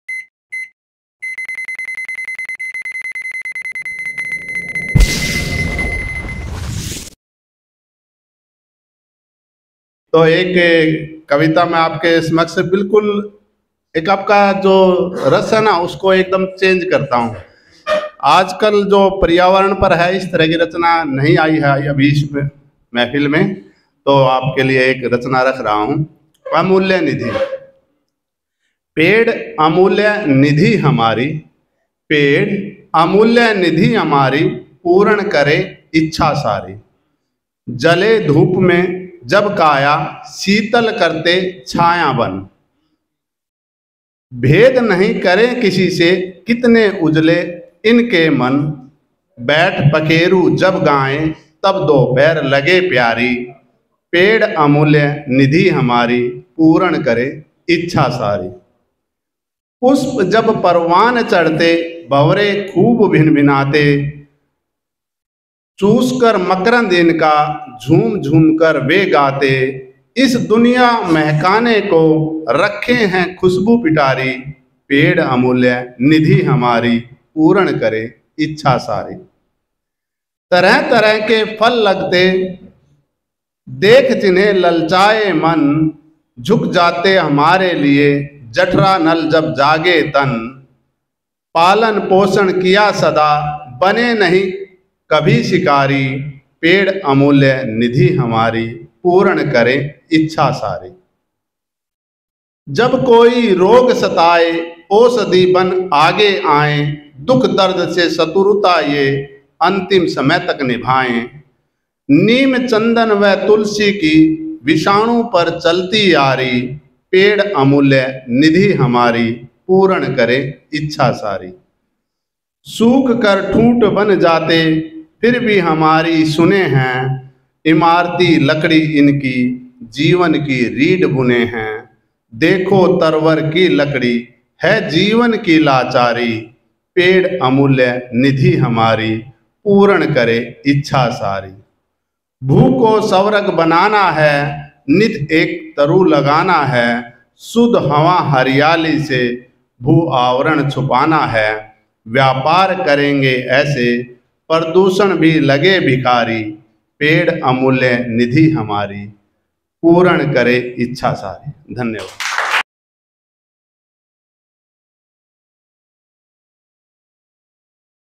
डॉo महेन्द्र सिंह सागर | सागरनामा साहित्य मंच | महात्मा गांधी व लाल बहादुर शास्त्री जयंती स्पेशल
Presented By

SagarNama is an open mic platform for Poets, Stand-up Comedians, Storytellers, and Musicians in Hisar, Hansi, Bhiwani,Tosham, Rohtak, Kaithal(Haryana).